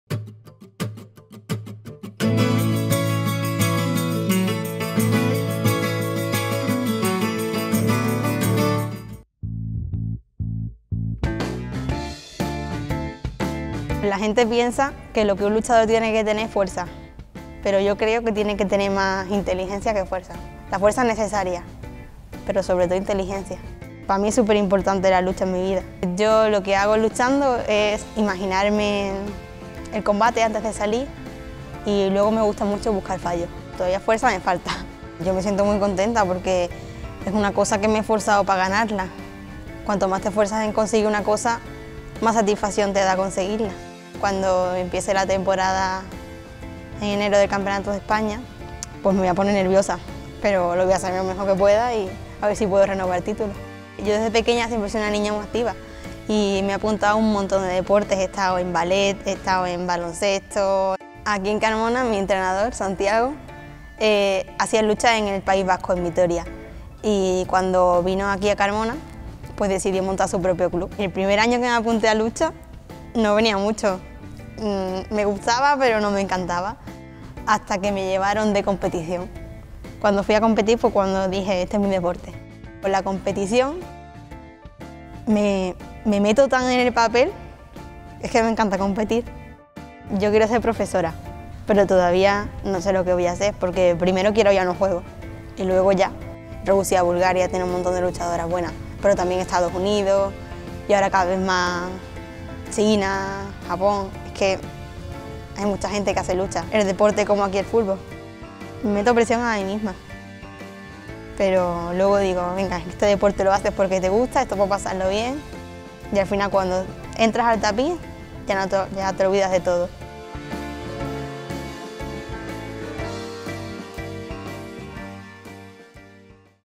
HDL Marina Rueda, la chica de oro de la lucha olímpica española
Marina Rueda es, con tan solo 21 años, campeona de España absoluta en lucha olímpica. La deportista, natural de la localidad sevillana de Carmona, tiene grabado a fuego un objetivo en su mente: clasificarse para los Juegos Olímpicos de Río en 2016. Cuando se le pregunta por la clave de los buenos resultados pese a su juventud, lo tiene claro: “Me encanta competir. En el tapiz me transformo”. En contra de lo que pueda pensarse, para ella, la principal cualidad de una luchadora debe ser la inteligencia.